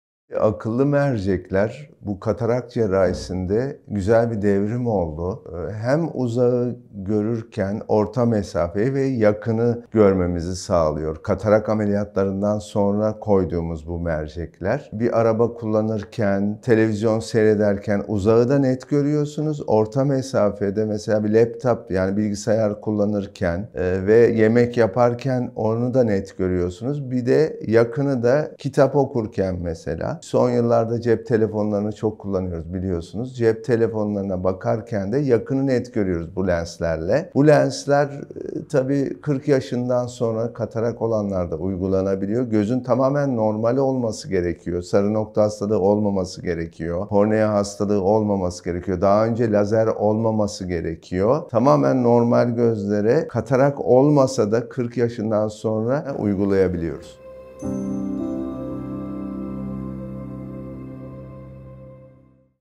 Akıllı Lens Nedir?
Denizli Özel Yaylalı Göz Hastanesi kurucusu Prof. Dr. Volkan Yaylalı Akıllı Lens ile ilgili önemli bilgiler veriyor.